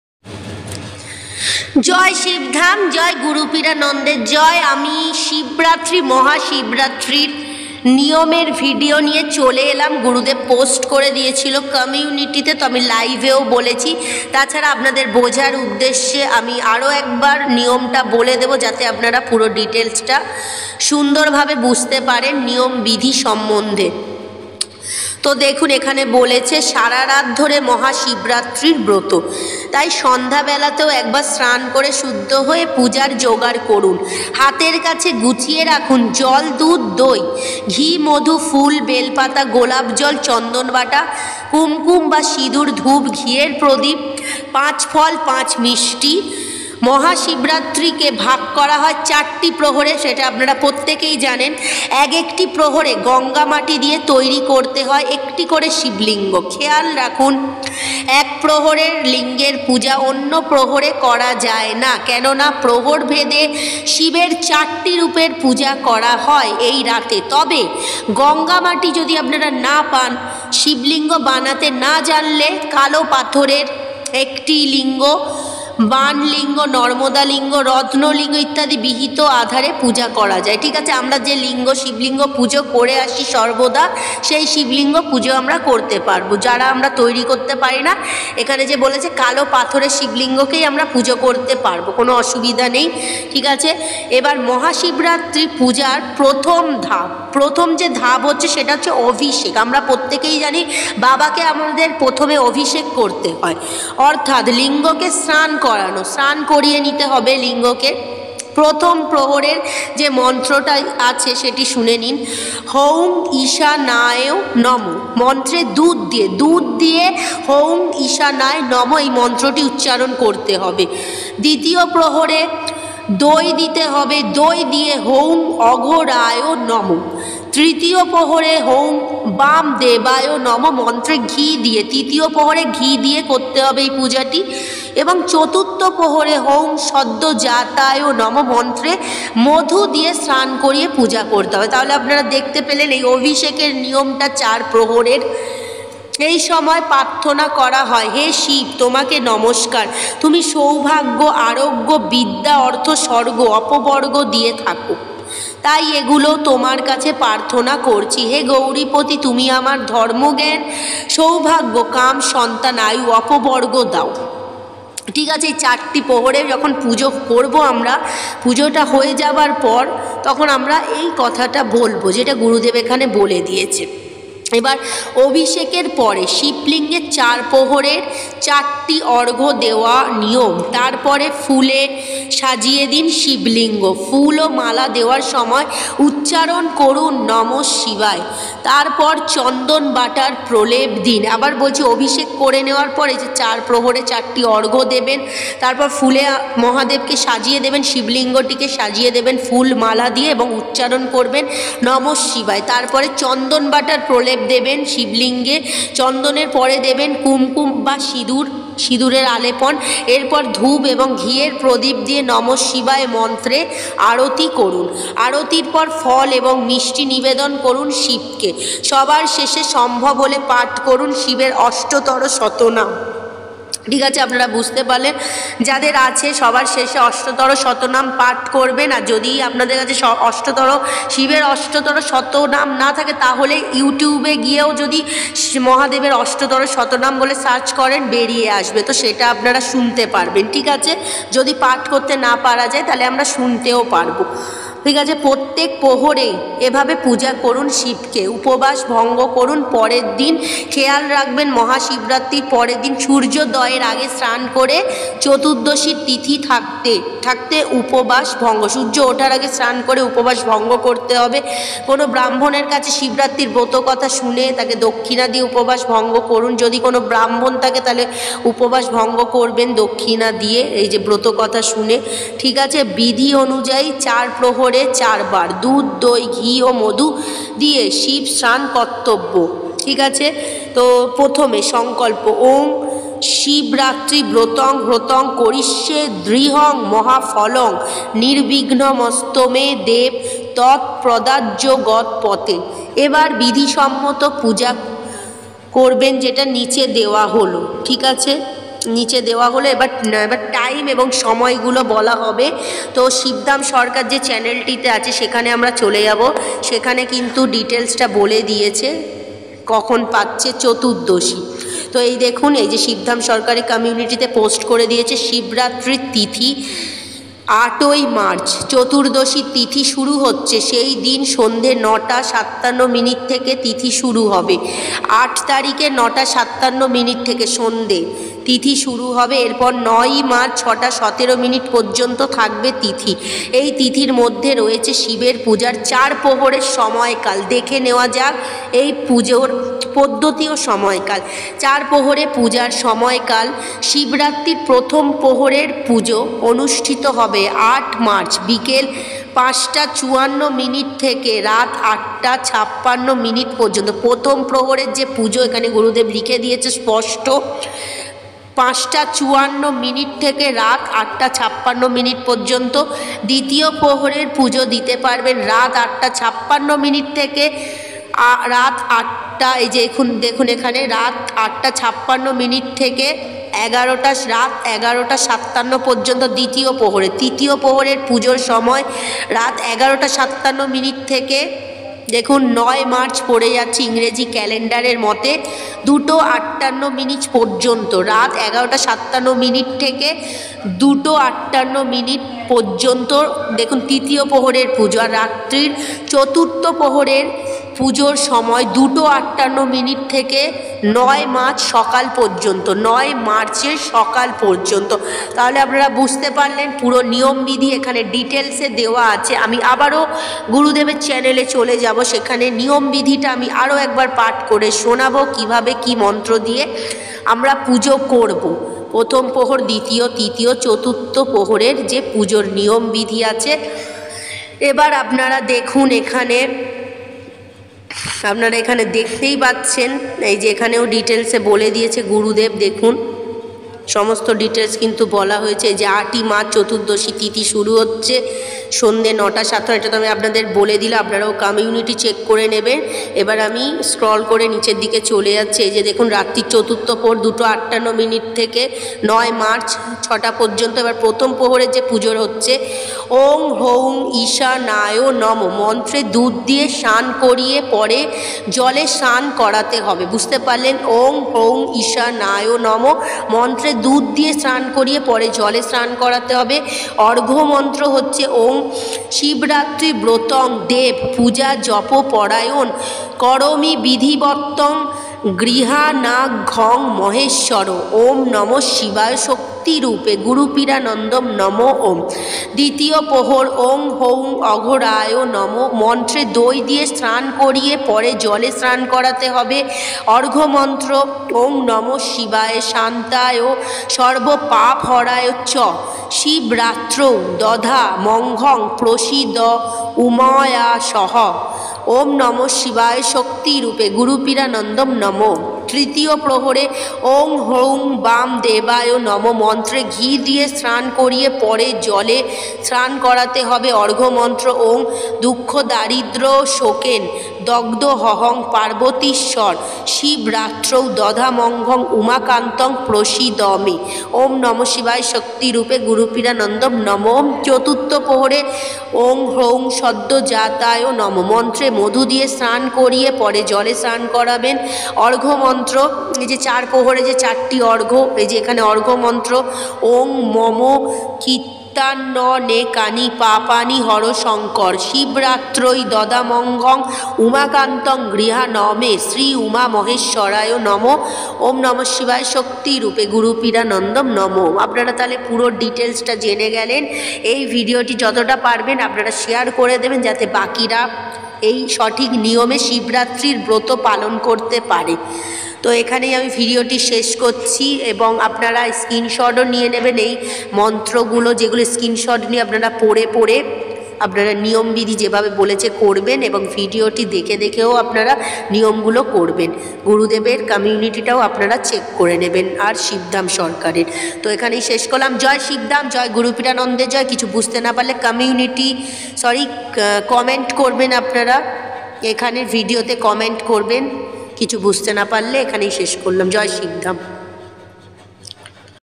মহা শিবরাত্রি পালনের সঠিক নিয়ম বিধি সম্পূর্ন দেখে নিন গুরুদেবের বলা নিয়ম মাফিক পুজো করুন
জয় শিব ধামের জয় জয় গুরু পীরানন্দের জয় 🙏🙏🌺🌺🌺🌿🌿
শিব ধাম এবার পশ্চিমবঙ্গ দুর্গাপুরে